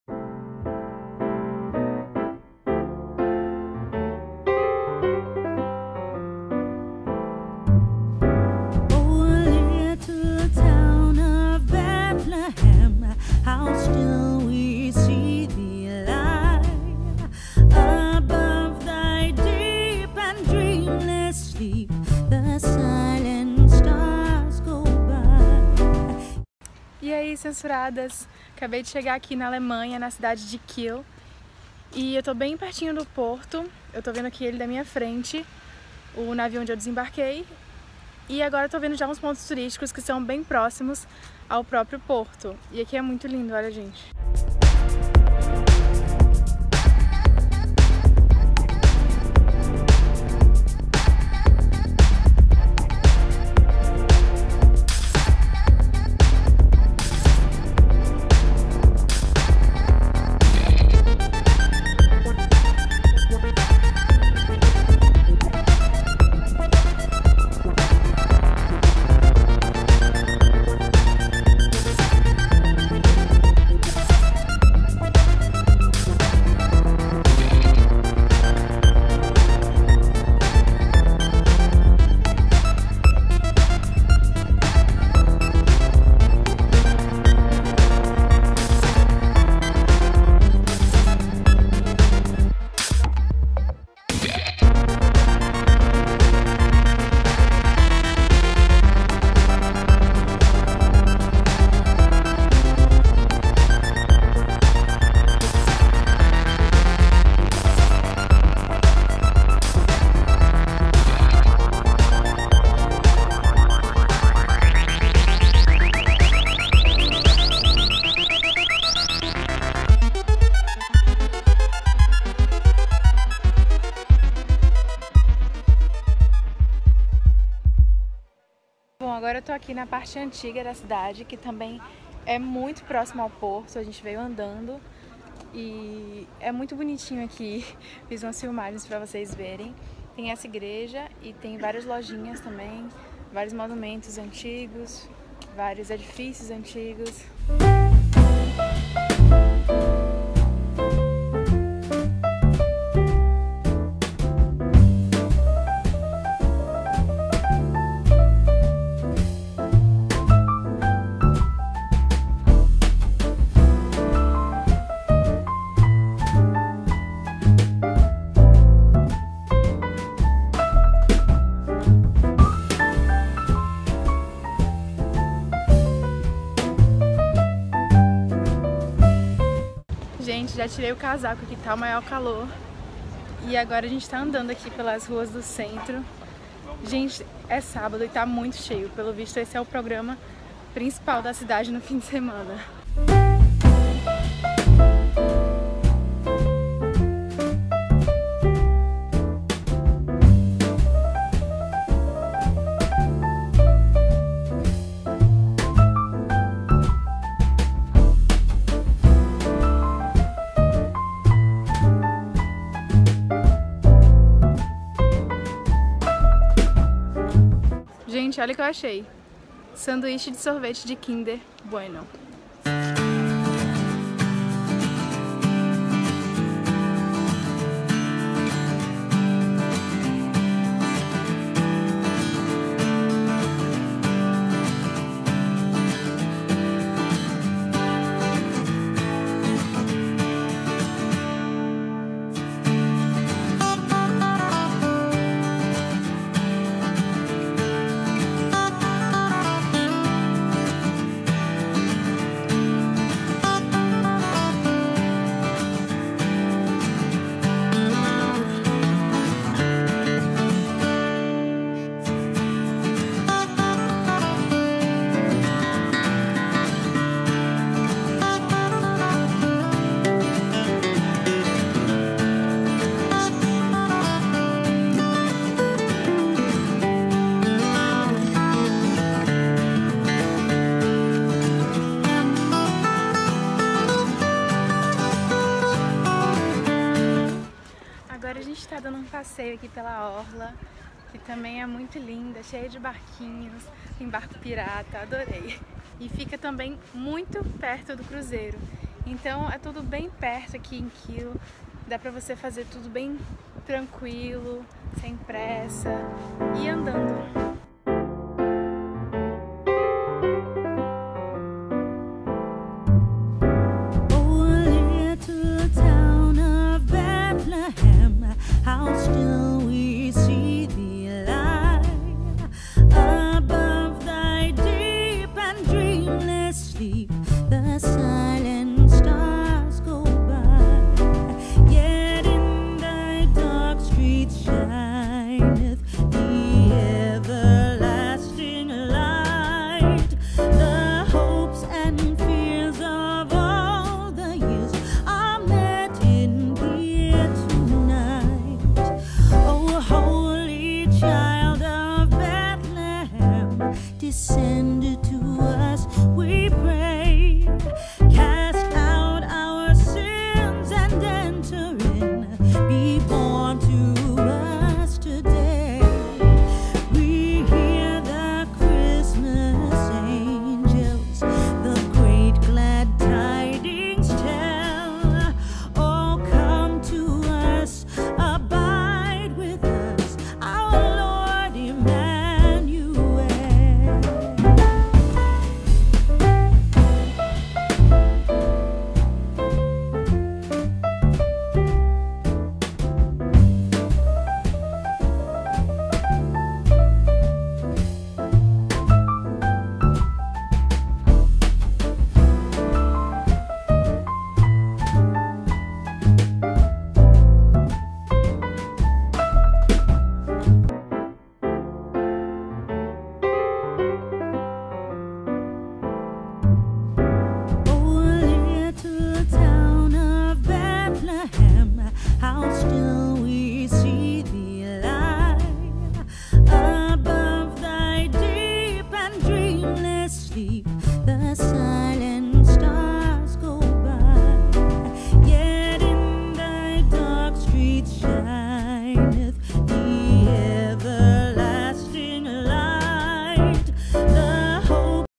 O charme de Kiel! | Alemanha
Kiel é a maior cidade do norte da Alemanha. A capital à beira-mar, localizada no fiorde de Kiel, foi quase totalmente destruída durante a Segunda Guerra Mundial. Ela é internacionalmente conhecida por hospedar a maior competição de vela do mundo, durante a Kieler Woche, além de ter o festival de música mais famoso do Norte! Durante a nossa visita, passamos por 2 passeatas: uma a favor dos direitos dos migrantes e refugiados e a outra foi a marcha da maconha.

Quer apoiar meu trabalho?

Mande algo pra mim:
CEP: 22793971
Rio de Janeiro - RJ
Victória Fechine

Canal: @canalcensuradas
Vicky: @vickyfechine